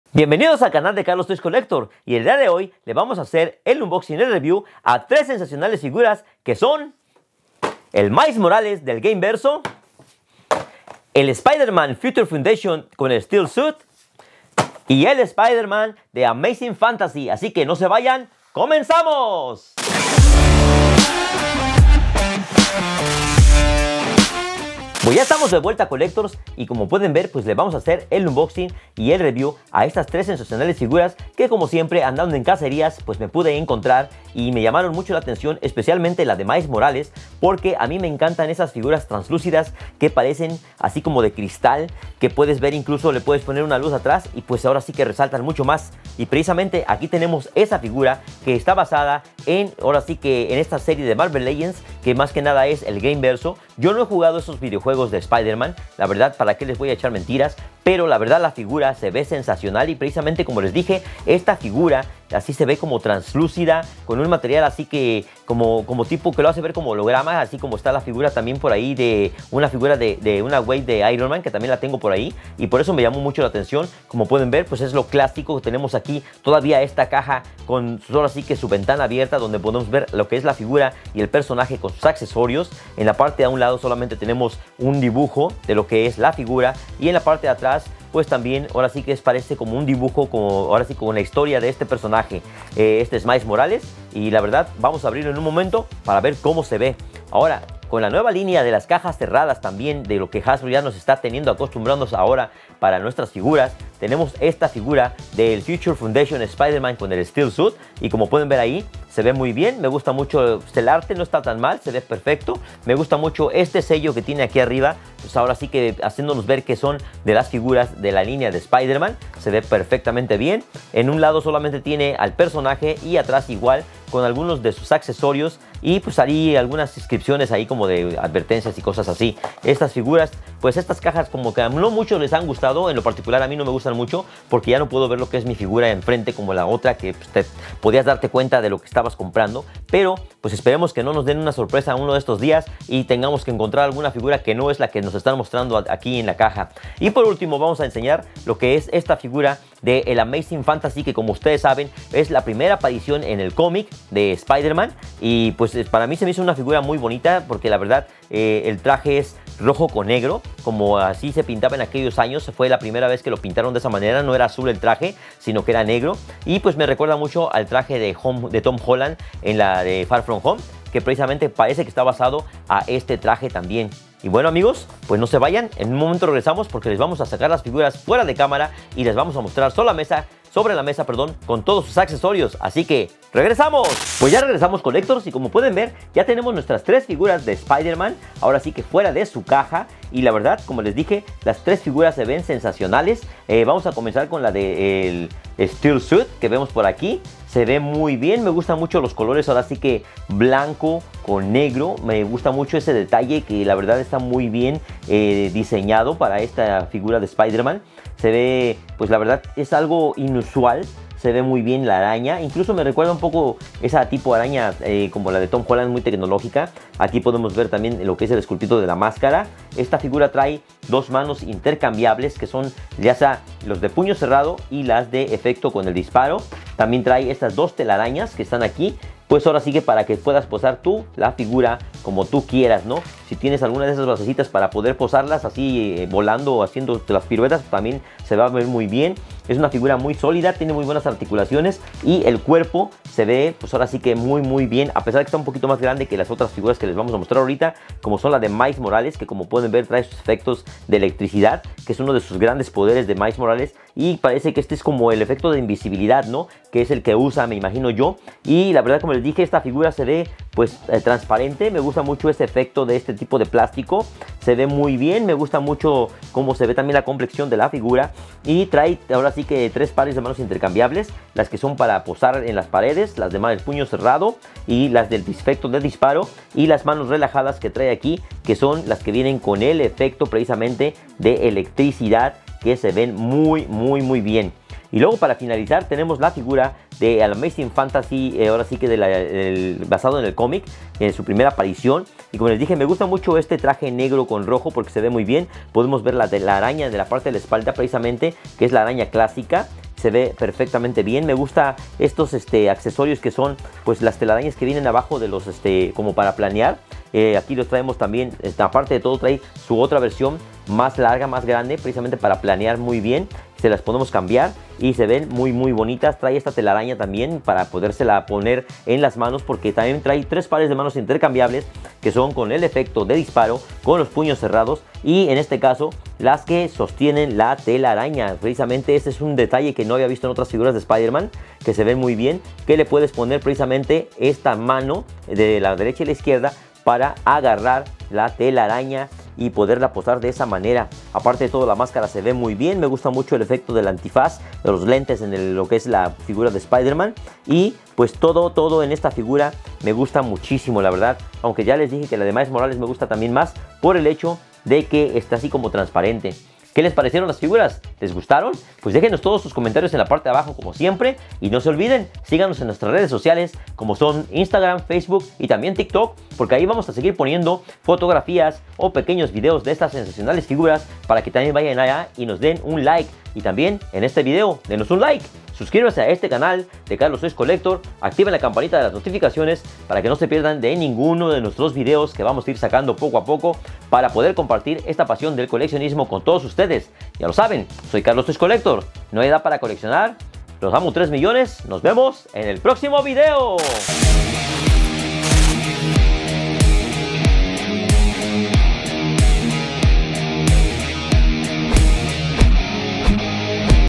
Hasbro Marvel Legends Spider-Man Fantasy, Spider-Man Future Foundation, & Miles Morales Exclusive
Hola Collectors hoy les traigo el unboxing y review de estas 3 figuras de la marca Hasbro Marvel Legends basadas en estas variaciones de nuestro amigo Spider-Man.

- Spider-Man Marvel Legends 60th Anniversary Amazing Fantasy Spider-Man 6-inch Action Figure
- Spider-Man Marvel Legends Future Foundation Spider-Man (Stealth Suit)
- Marvel's Spider-Man: Miles Morales Marvel Legends Miles Morales Exclusive

Dejen su comentario y díganme si tienen algúna de estas piezas o si planean buscarlas para su colección.